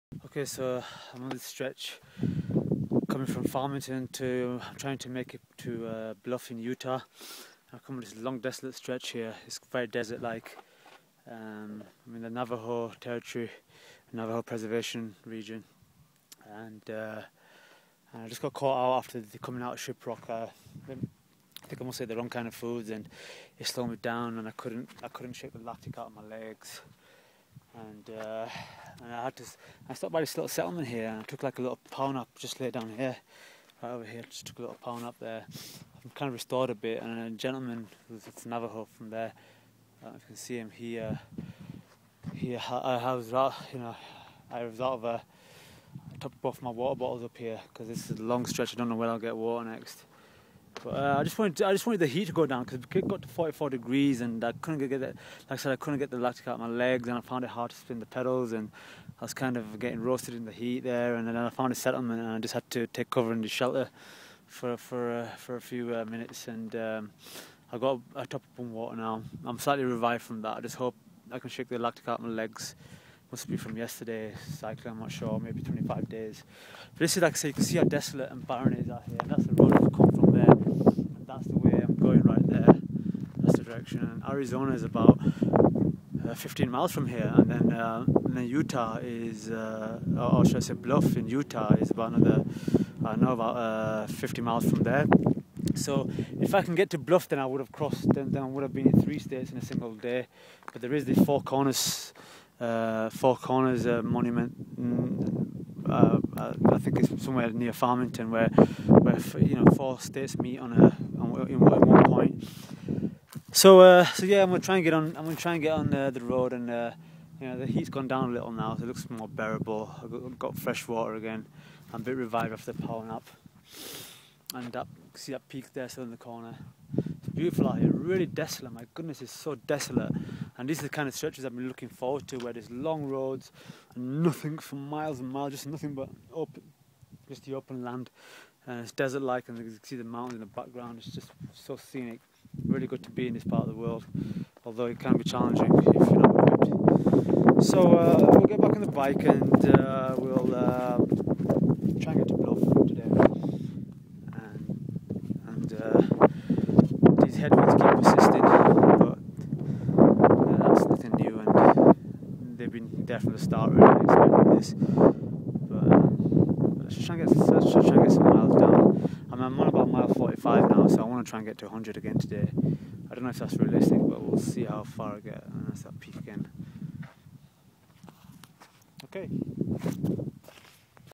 Imran's route 66 cycling challenge, day 26
Taking cover from heat and felt drained! Must be wrong food i ate